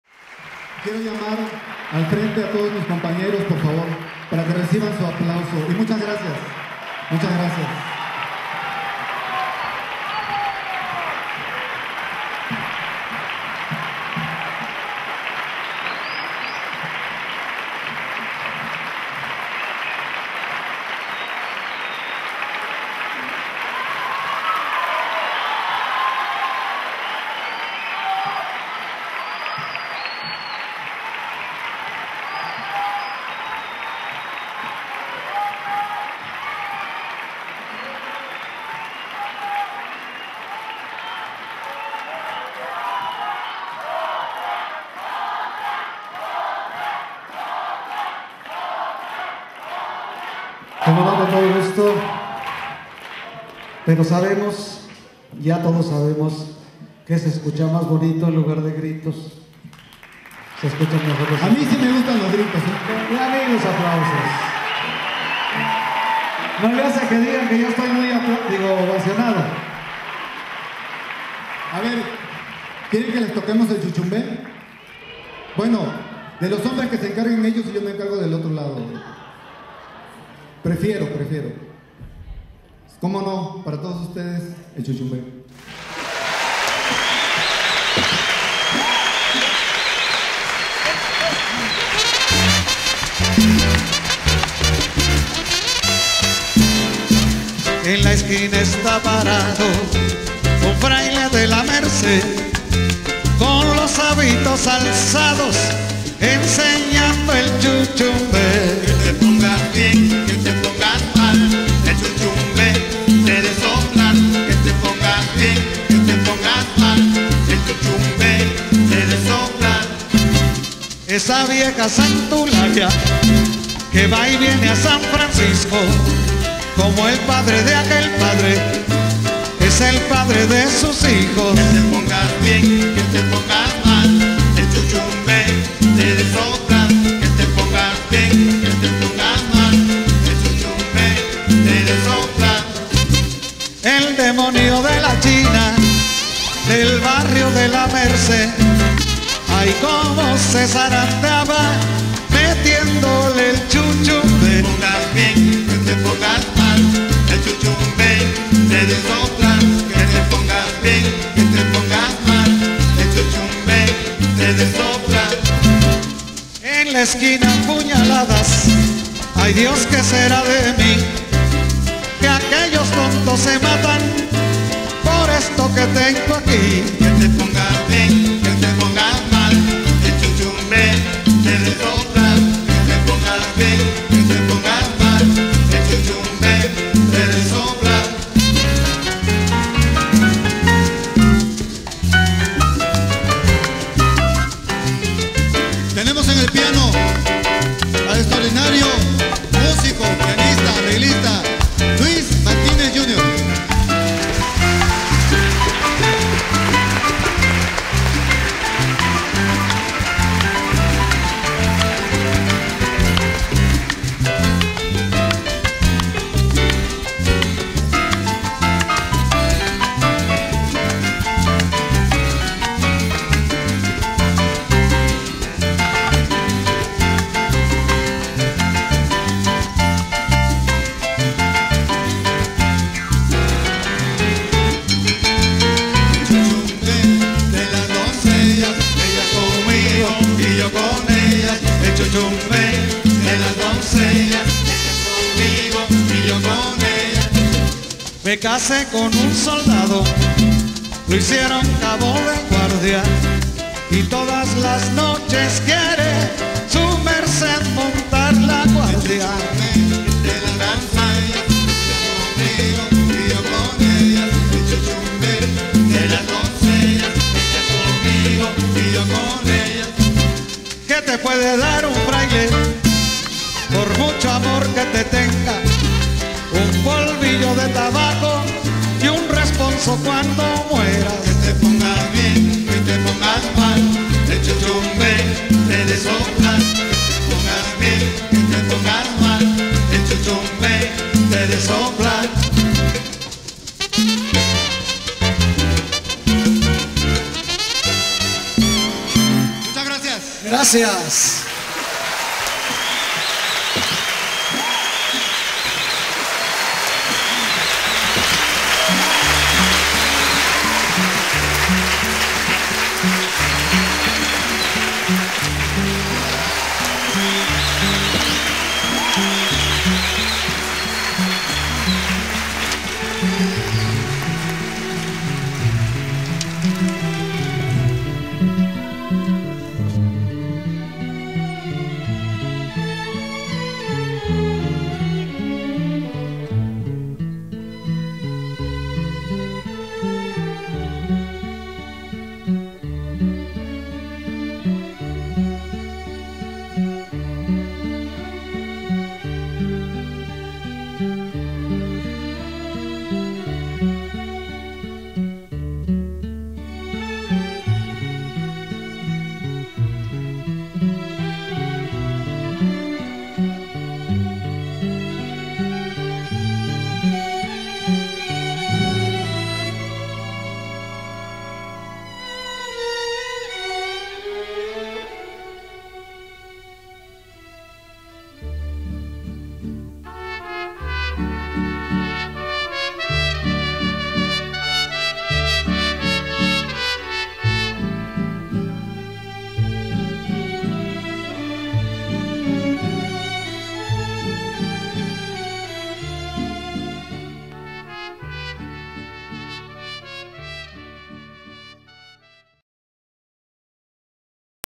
ENCORE El Caudillo del son en el Teatro de la Ciudad parte 17
México atravéz de sus canciones, Siglo XIX.
Concierto de Guillermo Zapata, El caudillo del son en el Teatro de la Ciudad
15 de septiembre de 2010. ¡Otro grito de independencia!
Músicos : Luis Martínez, piano; Violeta Ortega, jarana y voz; Paco Ocampo, Voz; Daniel Lantigua, trompeta; Alejandro Ocampo, percusiones; Juan Pablo García, bajo; Guillermo Henry, actor.

Investigación y dirección Guillermo Zapata